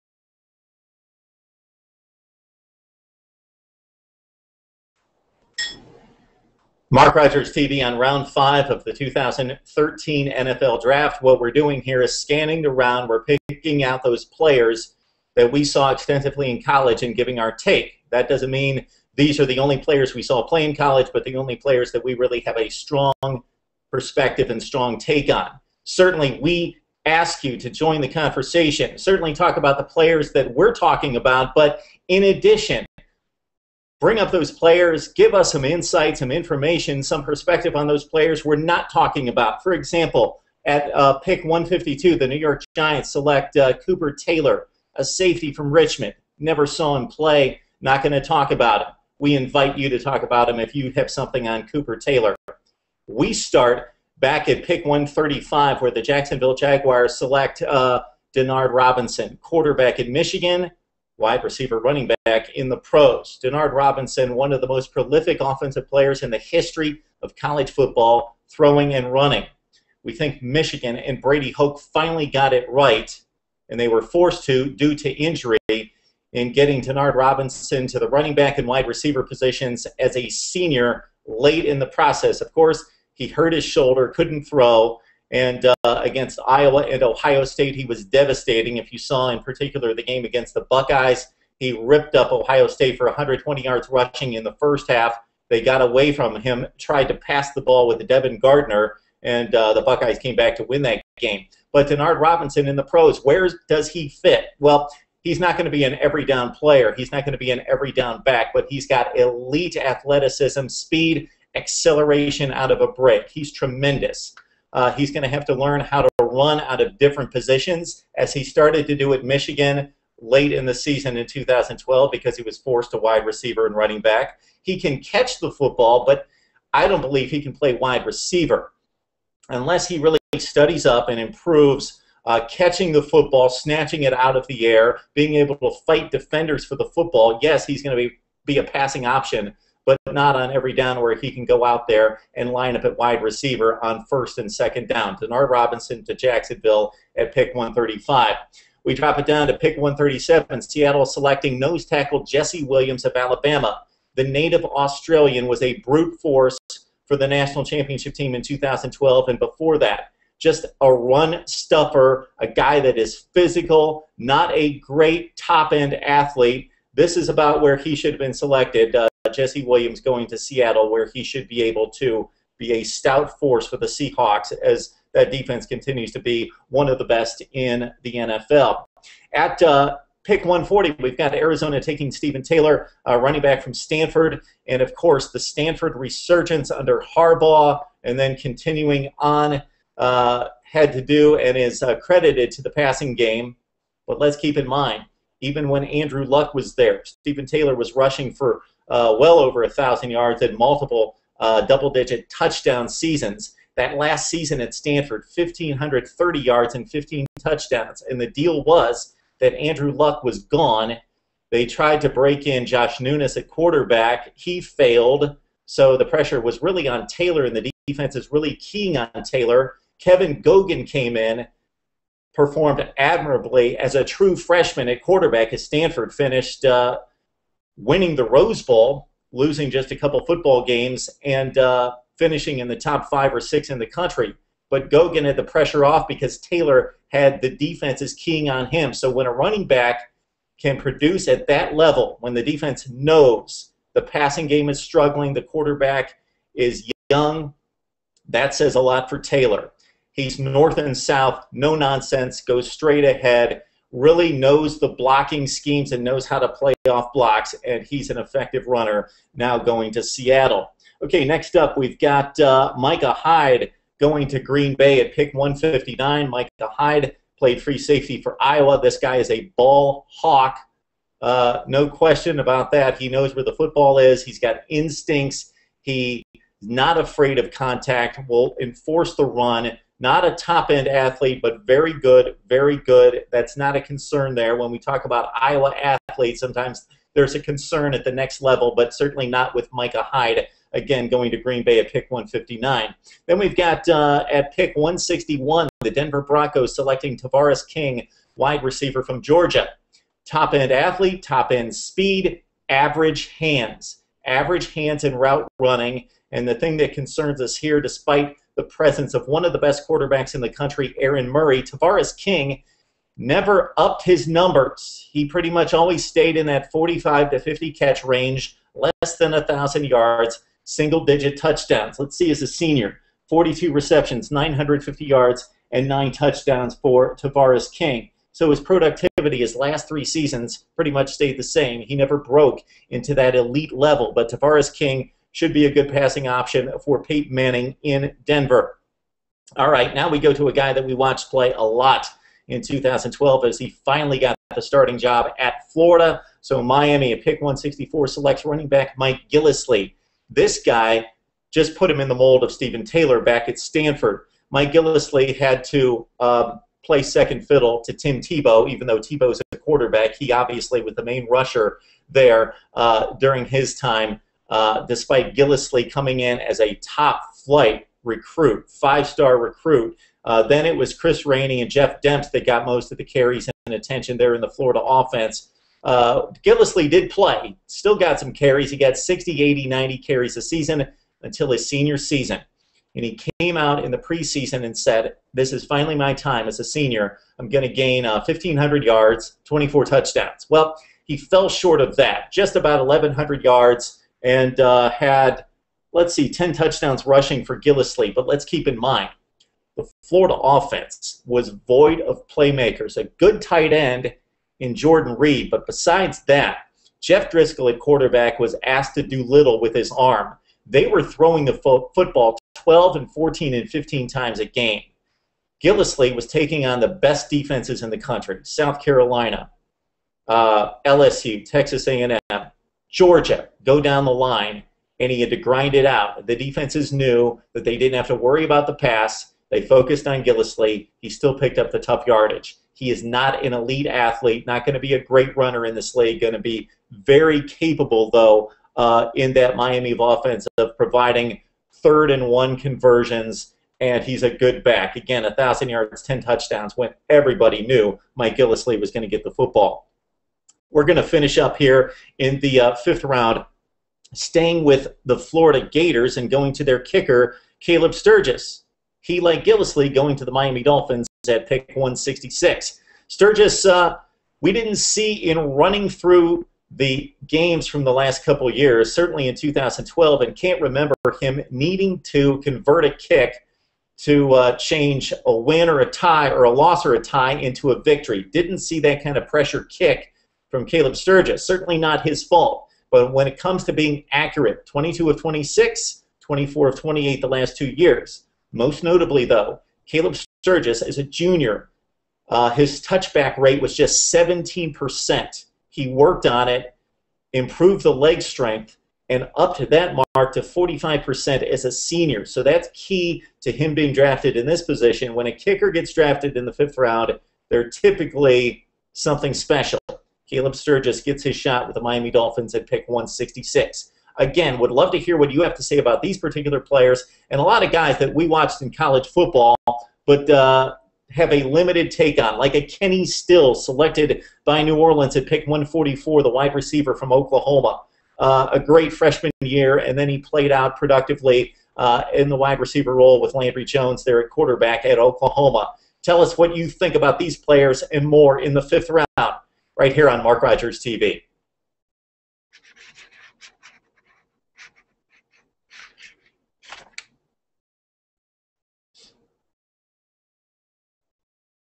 NFL Draft - 5th Round / Denard Robinson, Mike Gillislee, Stephan Taylor, Kaleb Sturgis
MarkRogersTV reviews the 5th Round of the 2013 NFL Draft - Denard Robinson, Jesse Williams, Stephan Taylor, Micah Hyde, Tavarres King, Mike Gillislee, Kaleb Sturgis